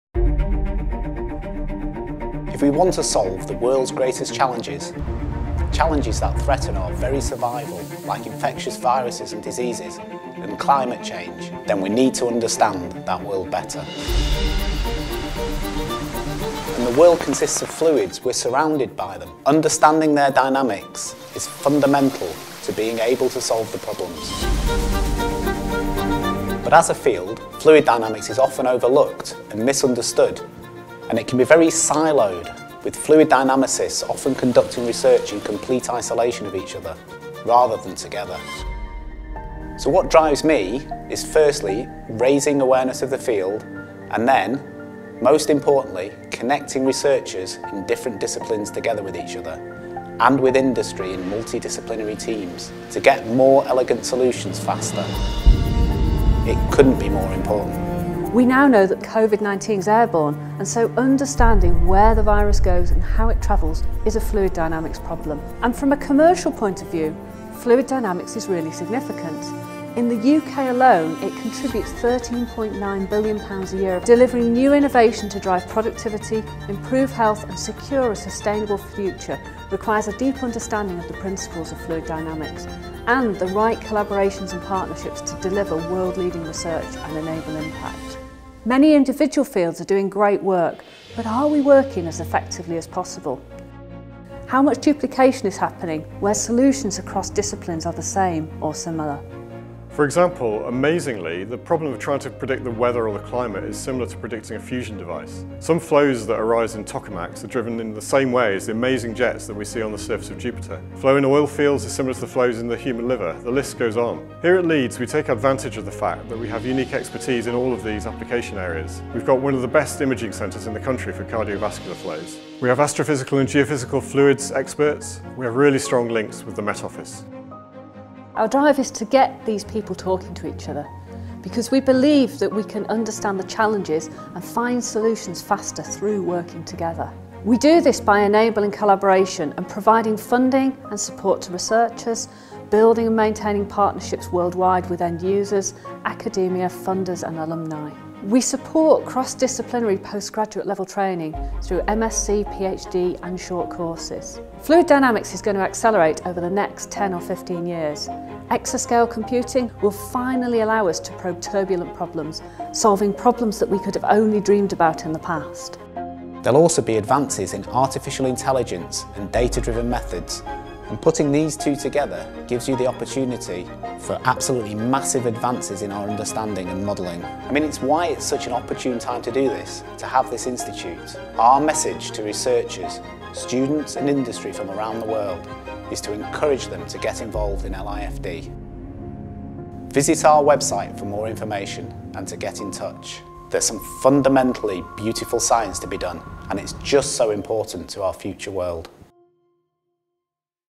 Introduction to LIFD
Fluids Dynamics lies at the heart of many societal and industrial challenges with practical application in the engineering and physical sciences and further into geophysical, astrophysical, environmental and biomedical regimes. The Leeds Institute for Fluid Dynamics is a cross-disciplinary research institute that brings together expertise in a collaborative framework in order to maximise the effectiveness and impact of fluids research.